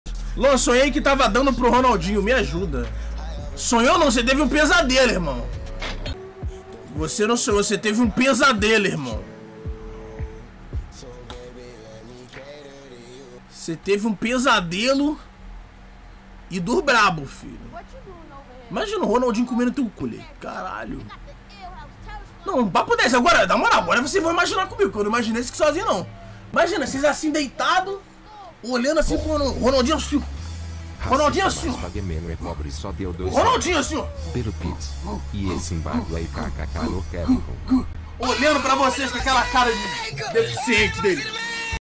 Imagina sonhar com o Ronaldinho te Comend0
clipe tirado da live do luangameplay.
inscrito sonhou com o Ronaldinho. Luangameplay manda a braba.